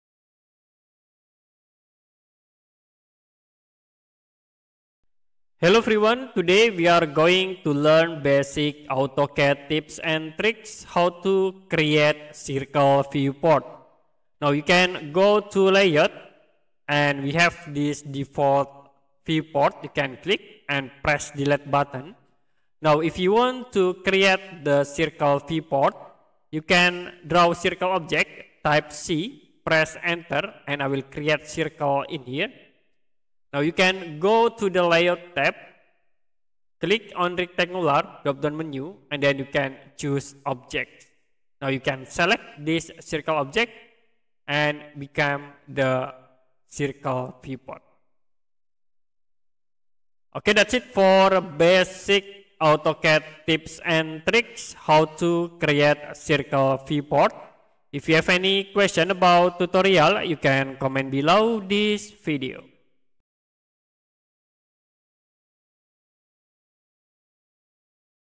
AutoCAD Tips 28 Circle Viewport
*Back*
*Next*
- ongoing

Learn essential AutoCAD tips for creating and managing a circle viewport. Enhance your drafting projects using these step-by-step guides.